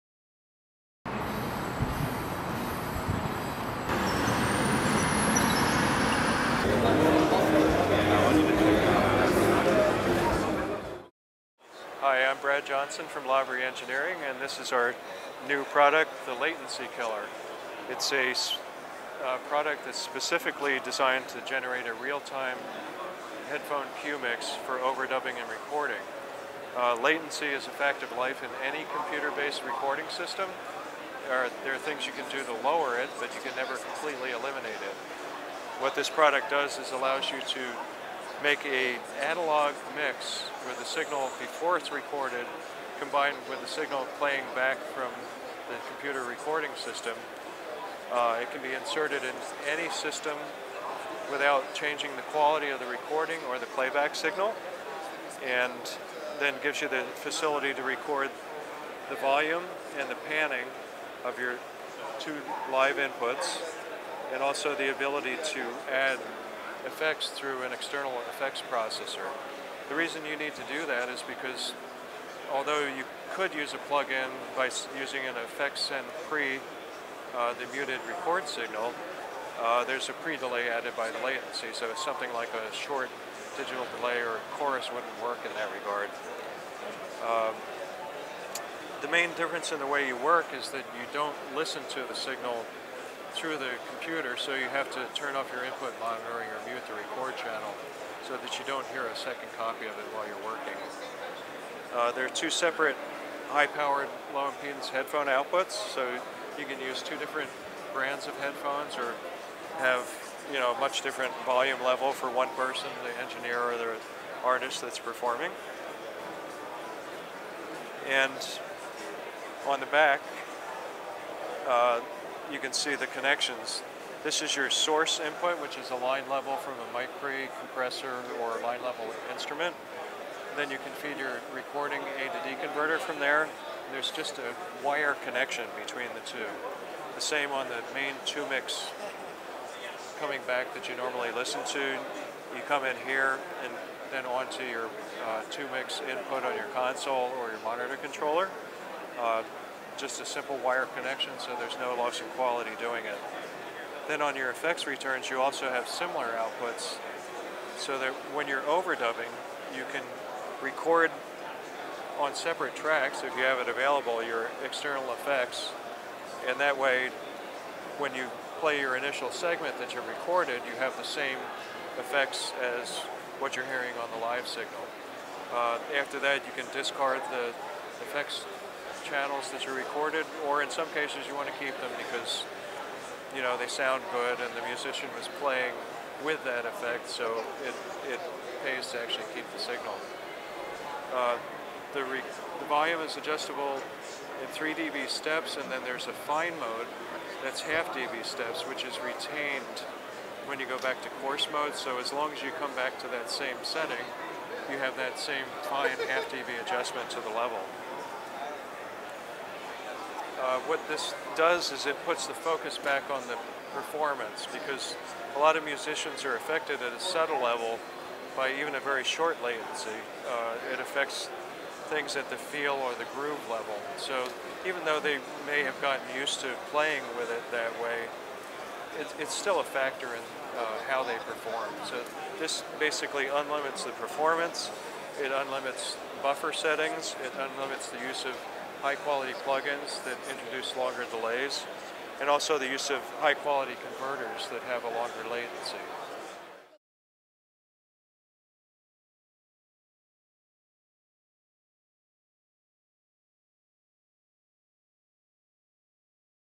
Vintage King @ AES: Lavry Latency Killer
Brad Johnson of Lavry Engineering talks about the new Latency Killer at AES 2012. The Latency Killer offers a big advantage over mixers: There is no signal alteration and no coloration because the LK-1 output is hardwired to the input (no electronics in the signal path). Mixers place many additional circuits in the signal path.